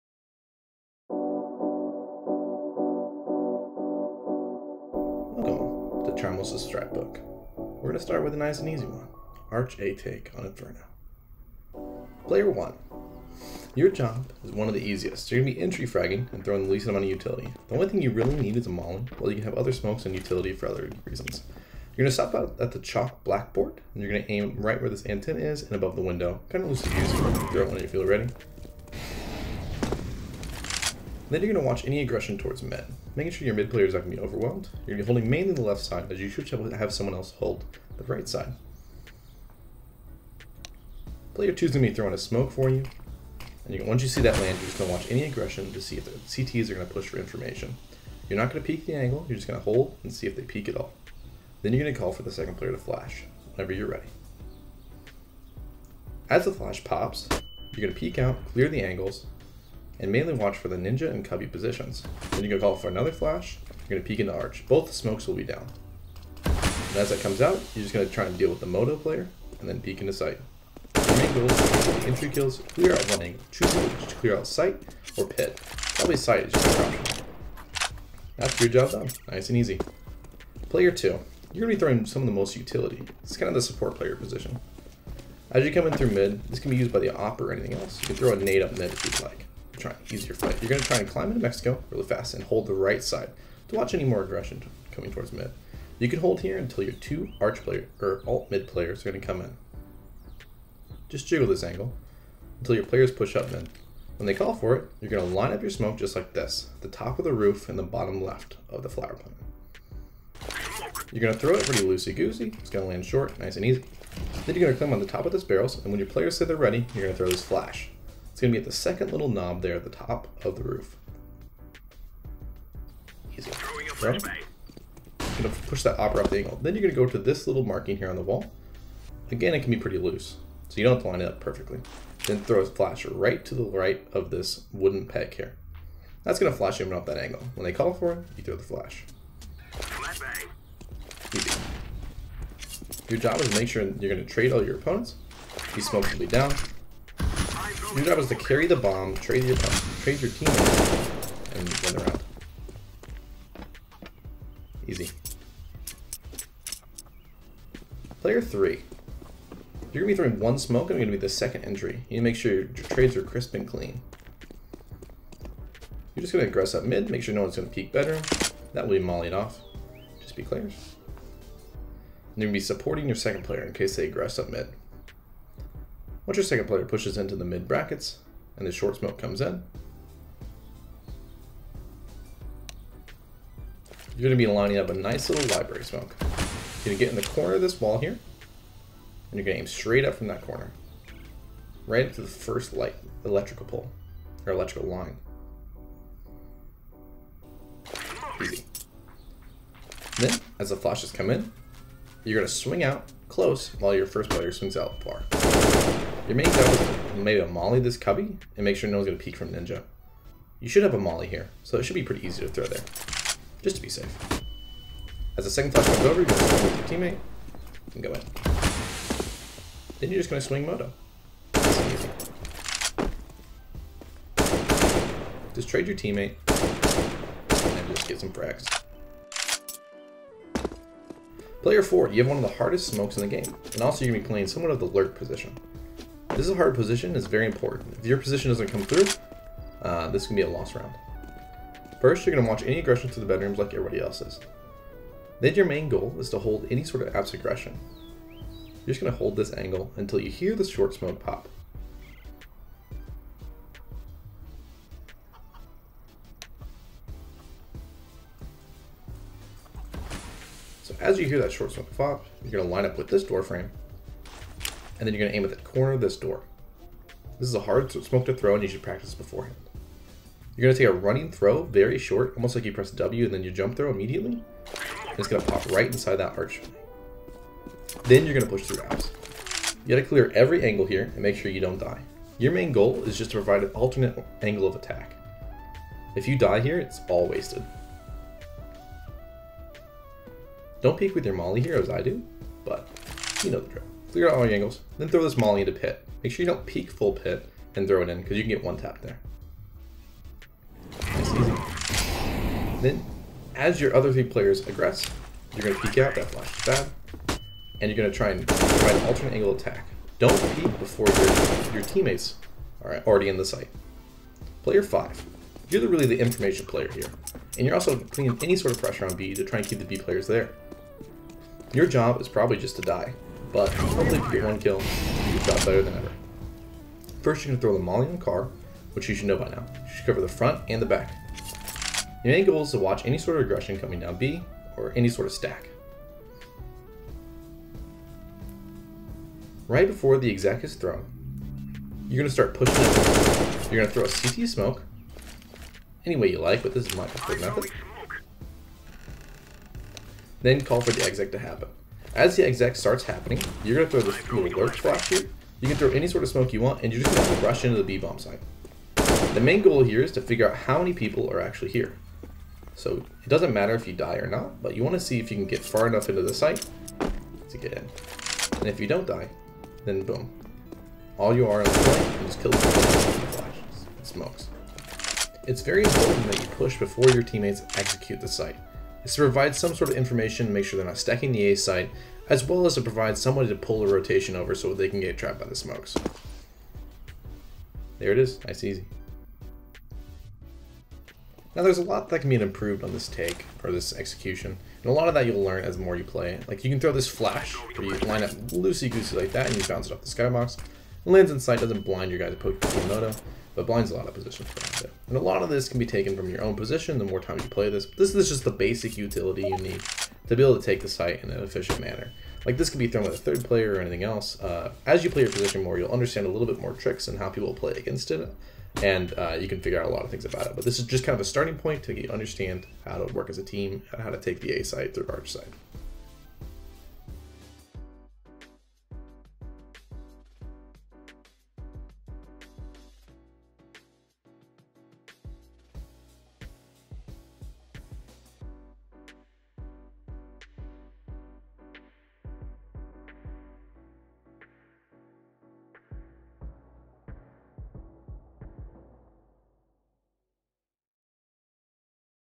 Inferno Arch A Execute
This is the first installation in the Charmless' Strat book. These strats are not complicated but go through each player's roles for an easy arch A execute. Please comment with any feedback or ideas you have.

Chapters
0:00 Intro
0:11 Player 1
1:19 Player 2
2:37 Player 3
3:51 Player 4
5:14 Player 5
6:27 Ending